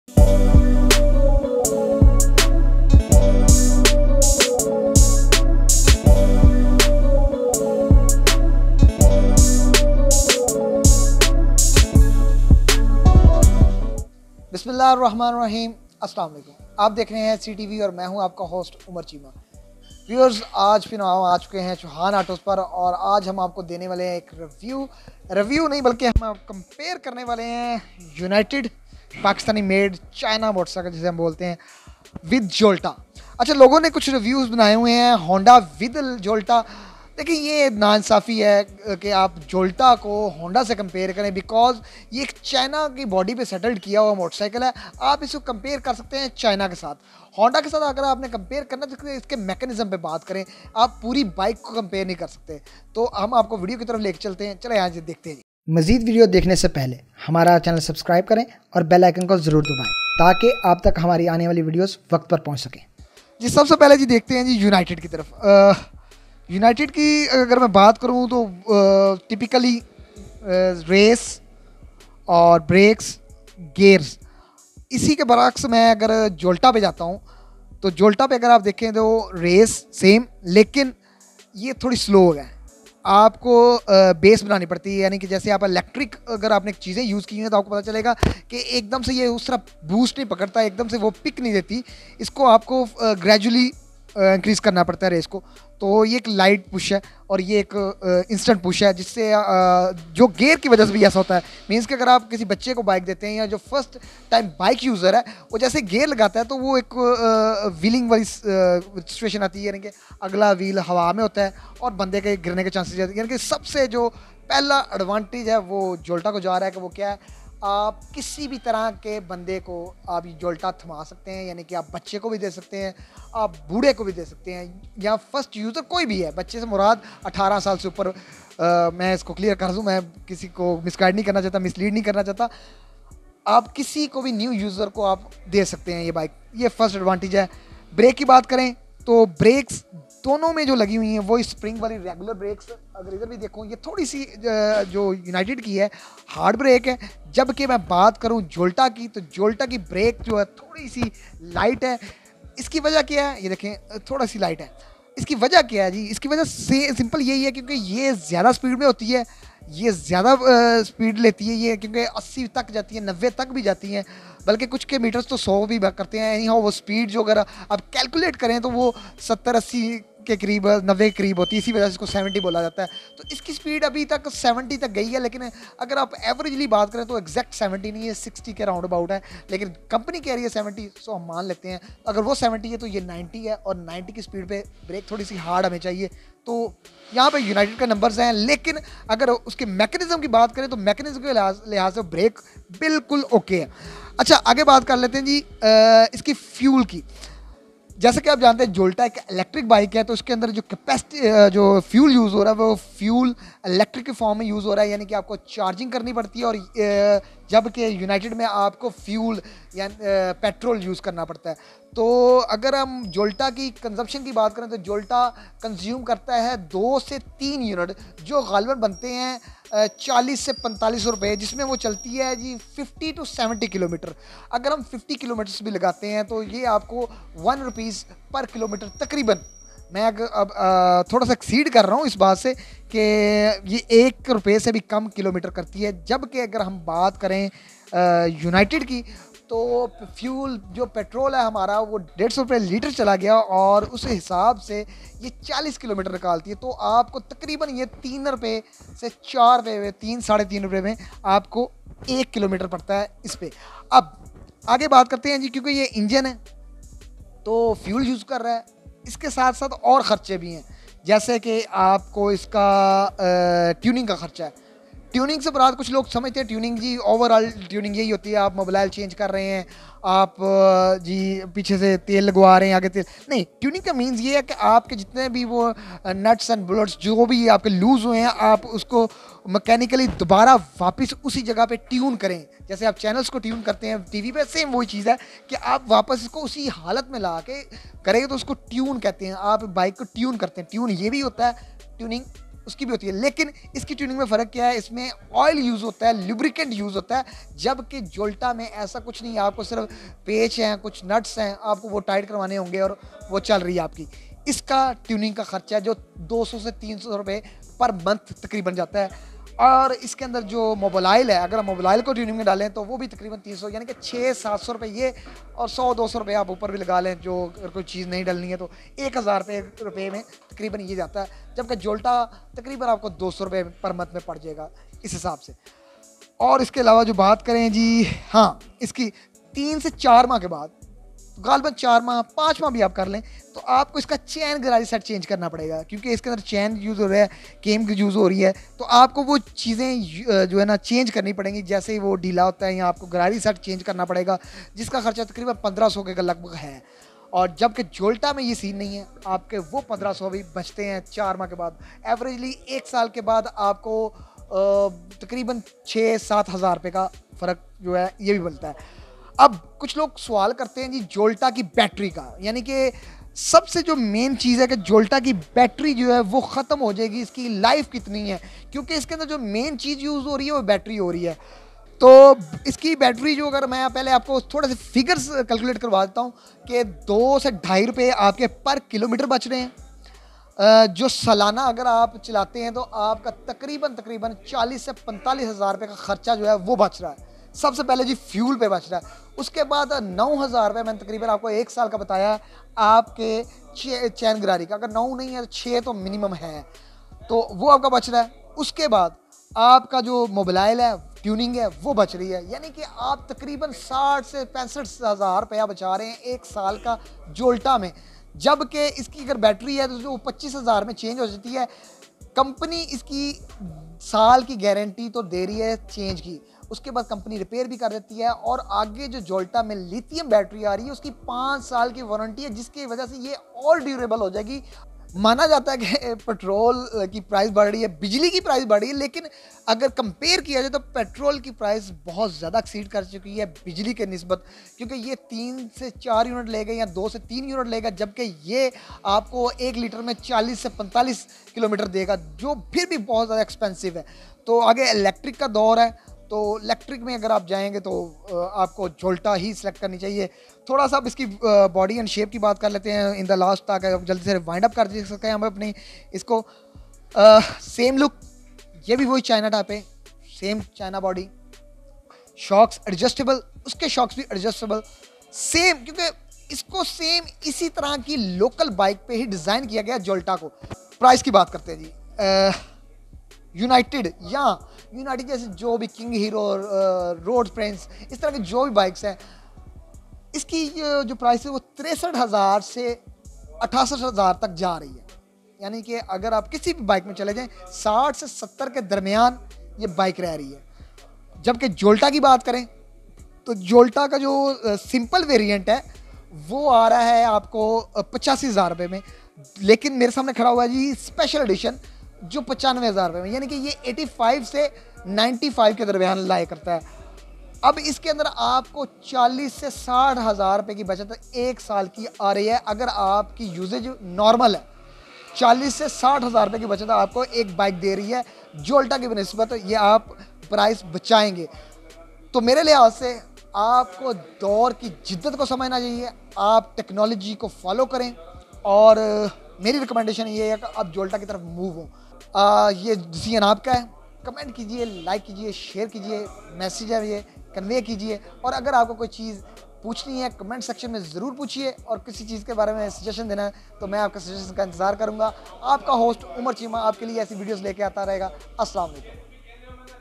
Jolta Electric JE-70 Vs United US-70 | Comparison Between Electric and Petrol Bikes | 2022 Models
Jolta Electric JE-70 Vs United US-70 | Comparison Between Electric and Petrol Bikes | 2022 Models | @SeeTVHDOfficial

In this Video you will get complete review of # 2022 models of Jolta JE70 and United US70. You can see its specification suspension and speed. You can also compare Electric Motorcycles with Petrol Motorbikes

Electric Bike leni chae ya petrol wali motorcycle dekhain muqamal comparison key sath video

Thanks for your Support and Visiting :-)... Team See Tv HD